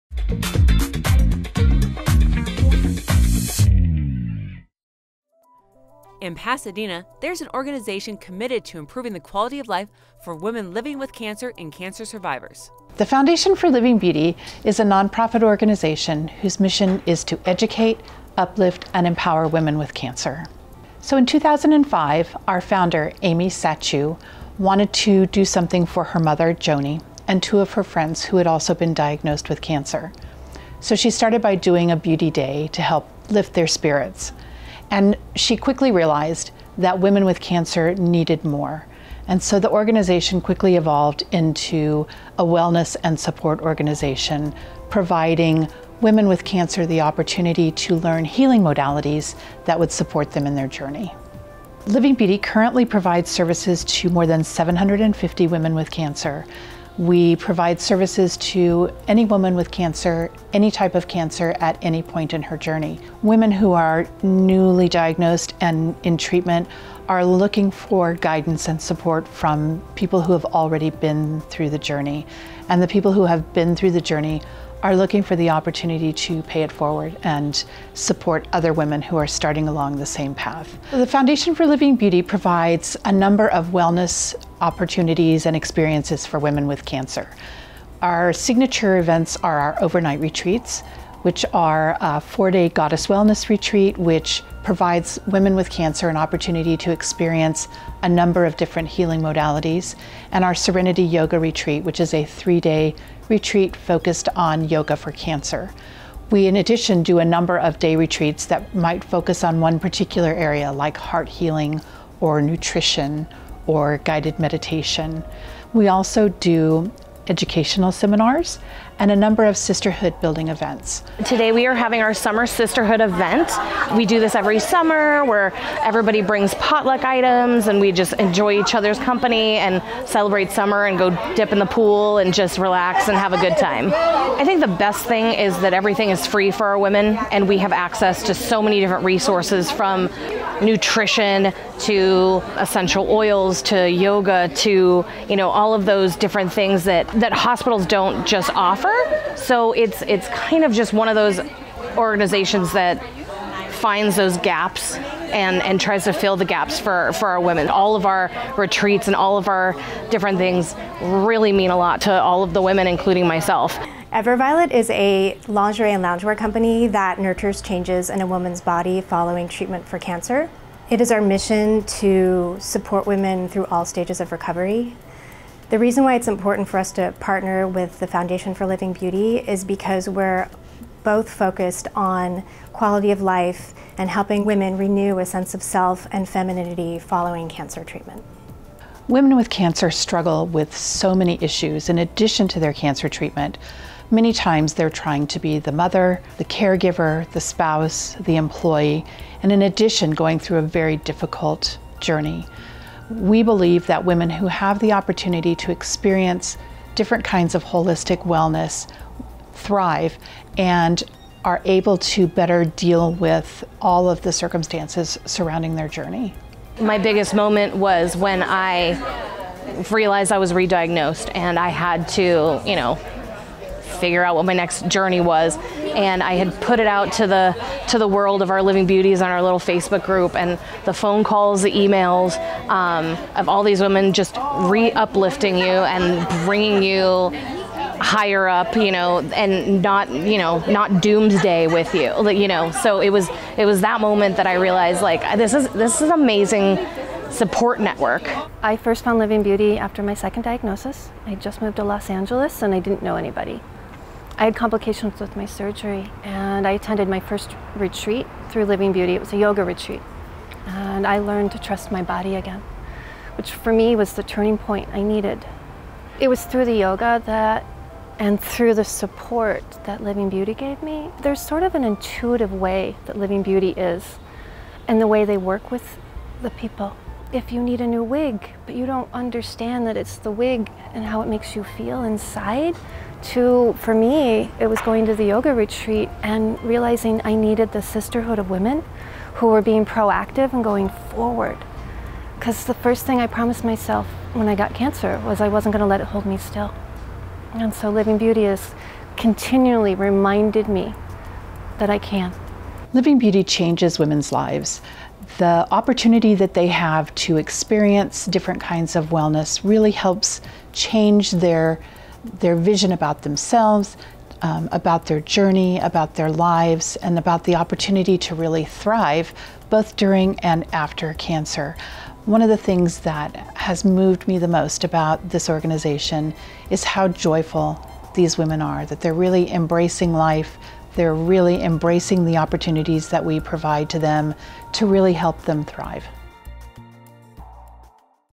What's Up Pasadena!: The Foundation for Living Beauty
Th e Foundation for Living Beauty provides free support services to women living with cancer through wellness retreats, educational workshops, and support events.